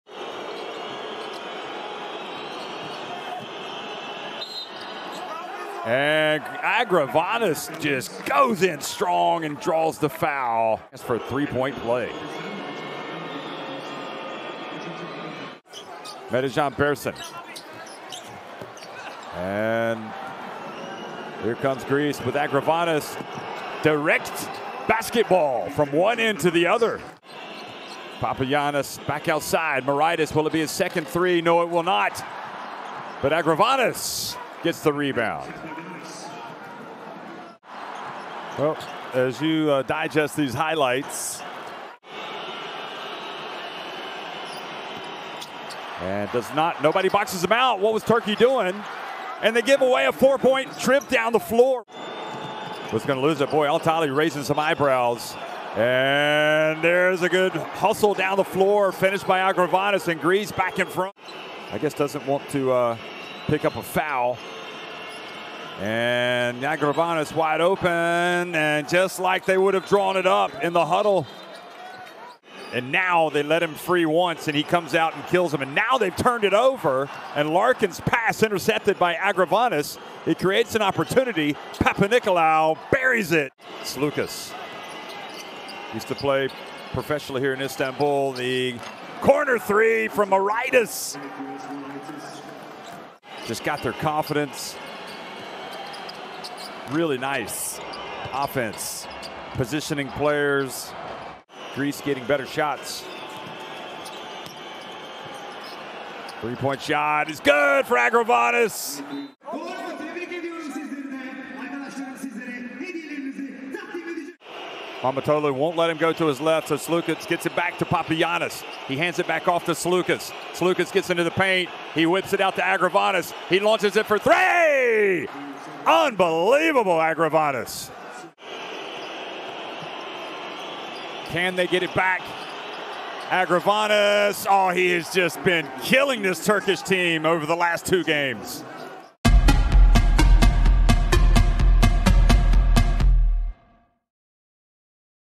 Dimitrios Agravanis 22 PTS & 11 REB for HELLAS | FIBAWC 2023 Qualifiers
Dimitrios Agravanis was incredible! He hits 22 points and 11 rebounds for Hellas! Enjoy the amazing performance by Dimitrios Agravanis for Greece against Turkey from the FIBA Basketball World Cup 2023 - European Qualifiers!